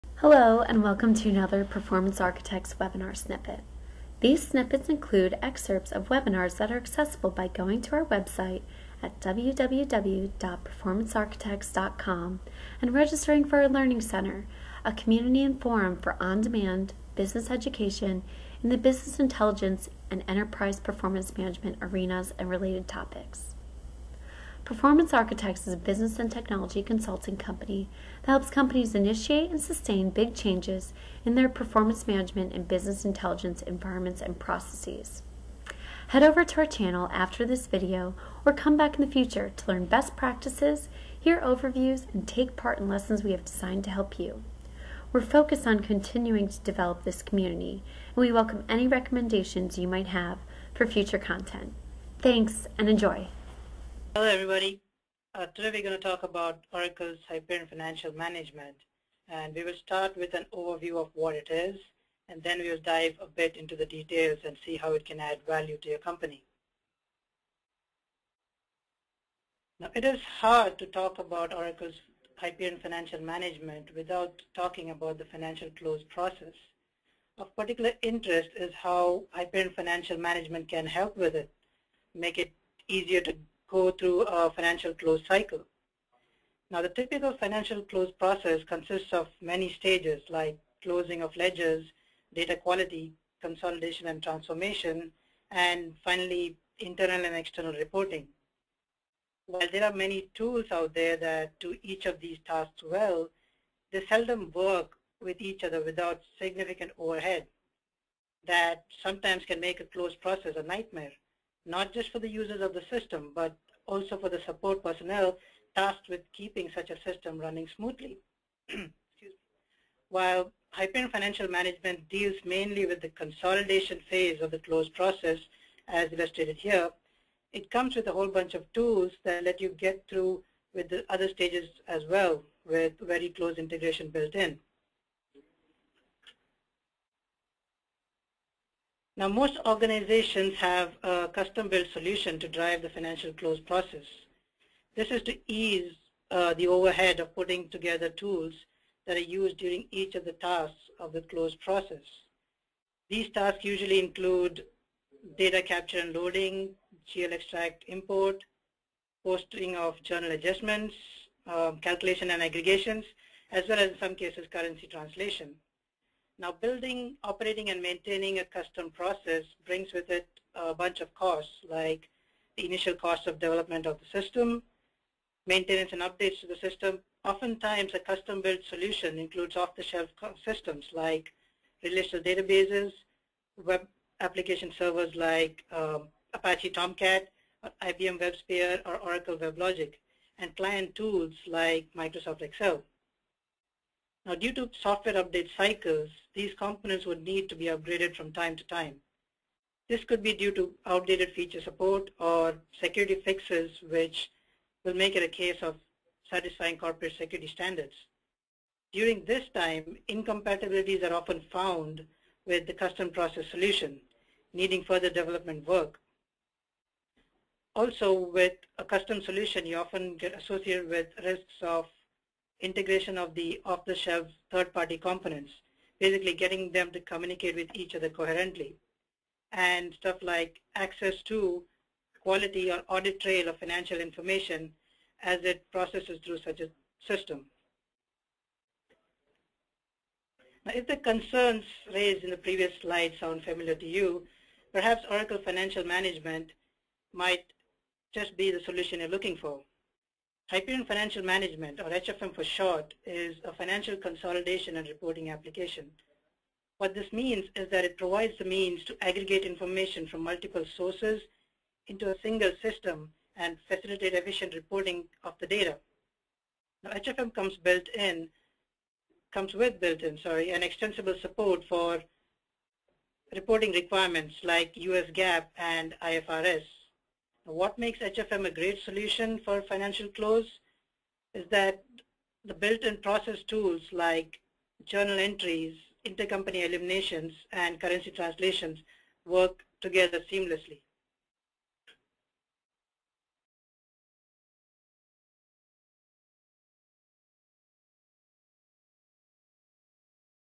"Introduction to Oracle Hyperion Financial Management (HFM)" Webinar Snippet
During this webinar, we will introduce you to Oracle Hyperion Financial Management (HFM) to streamline your financial close process and much, much more!  This webinar is suitable for Finance, Accounting and IT participants.